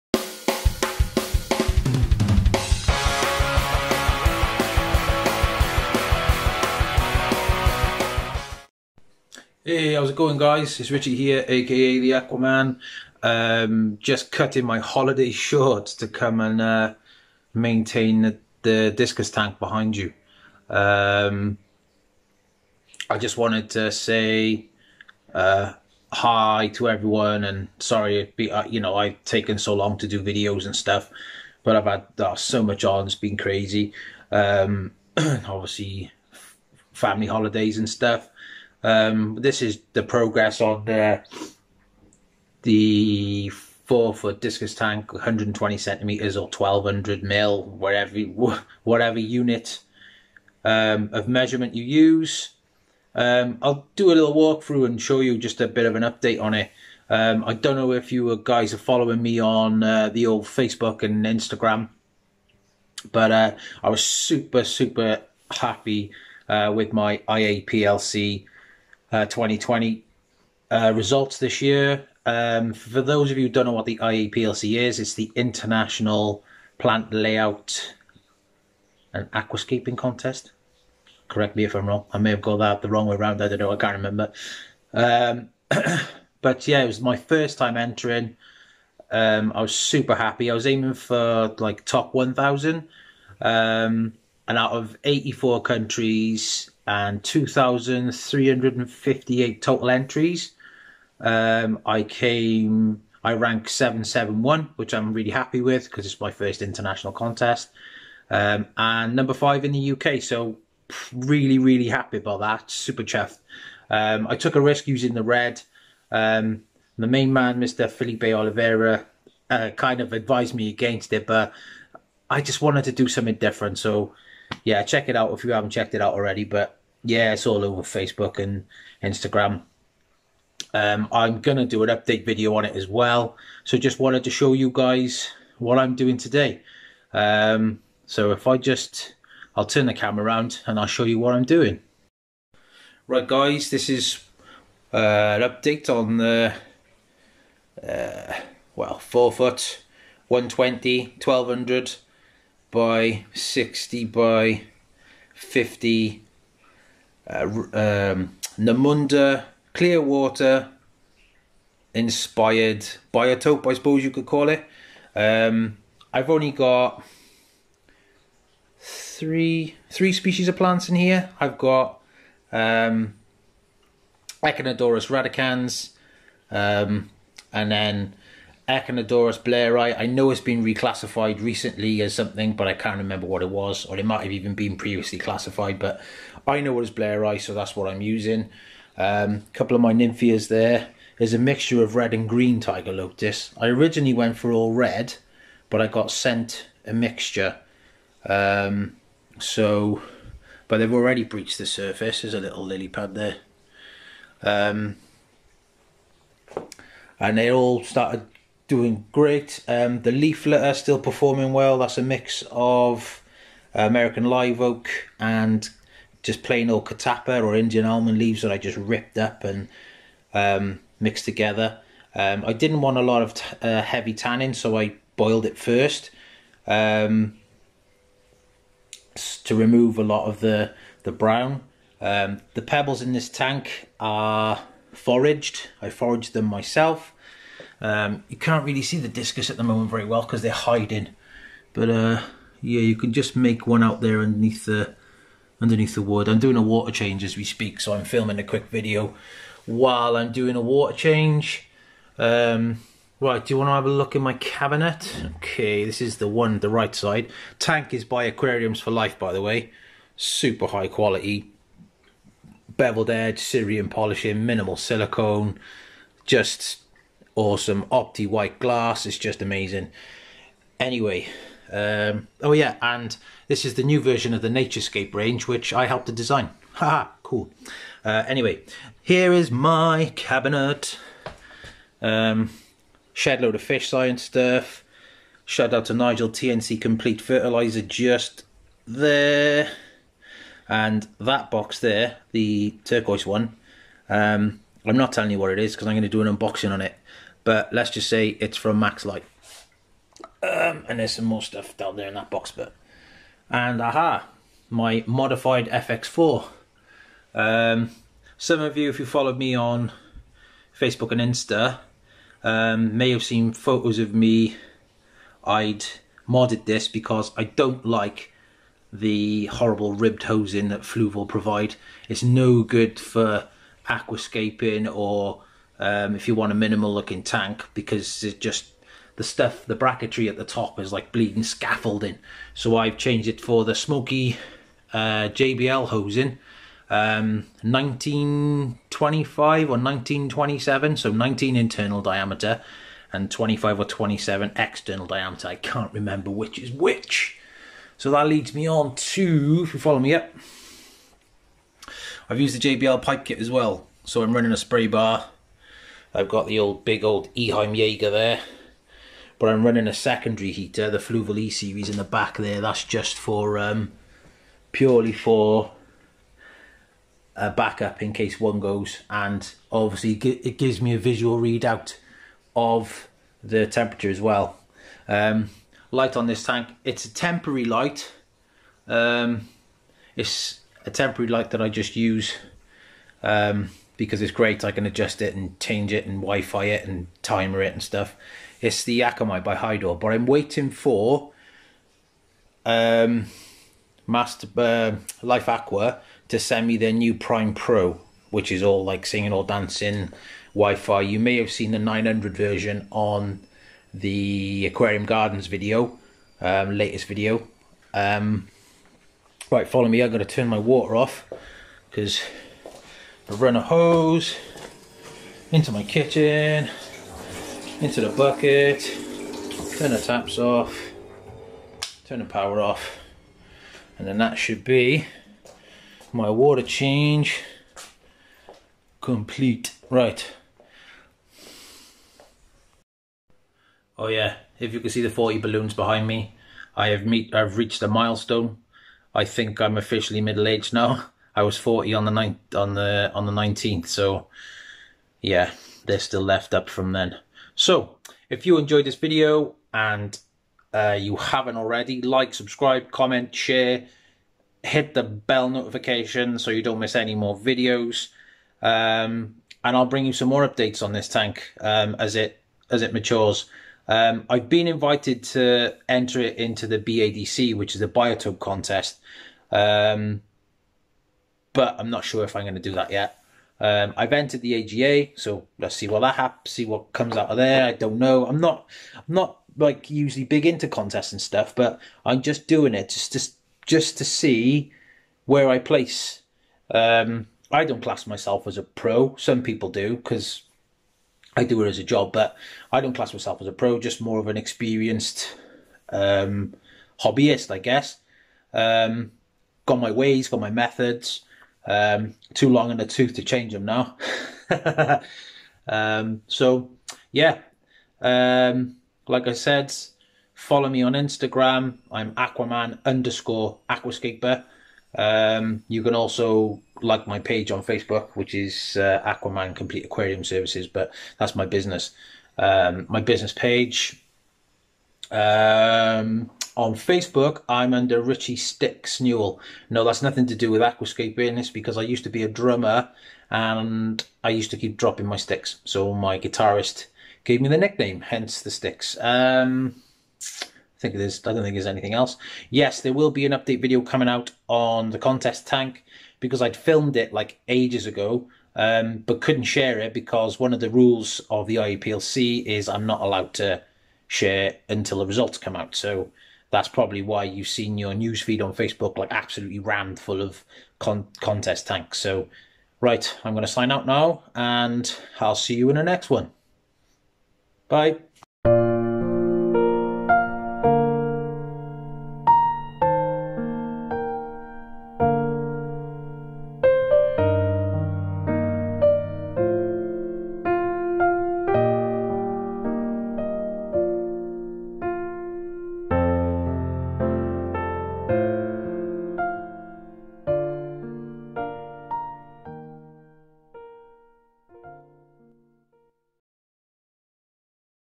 DISCUS AQUARIUM (UPDATE 2020)
This video is an update on my 360ltr 120x60x50cm or 4ftx2ftx20" 1200x600x500mm
This tank is based on a clearwater biotope.
In this case it's the Nhamunda region of the Amazon.
I originally wanted to use blue Heckel discus in this aquascape but they were unavailable so I've opted for turqs as they were one of the first domestic strains created from wild discus.

I got 10 4" fish from Chens Discus